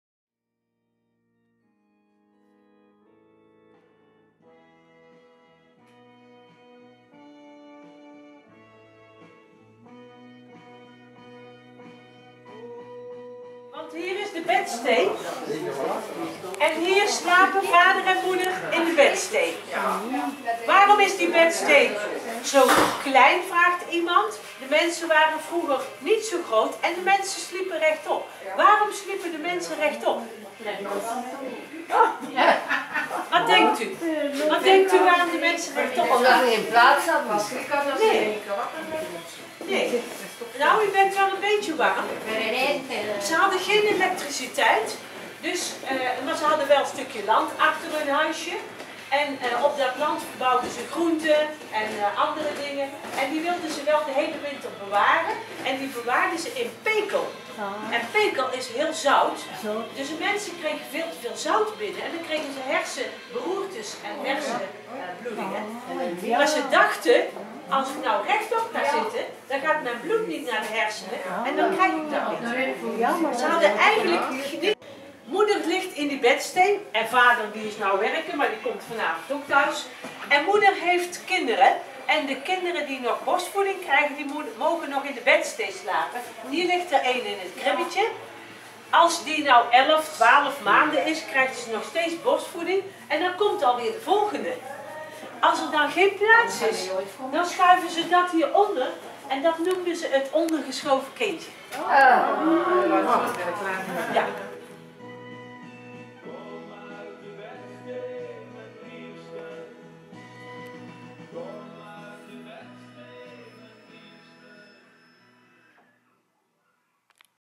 Brabants Museum Oud Oosterhout "Bedstee"
Vroeger dachten ze wanneer we rechtop gaan slapen er geen bloed in de hersenen zou komen kwam en dan was er minder kans op een beroerte. Het gebruik van te veel zout.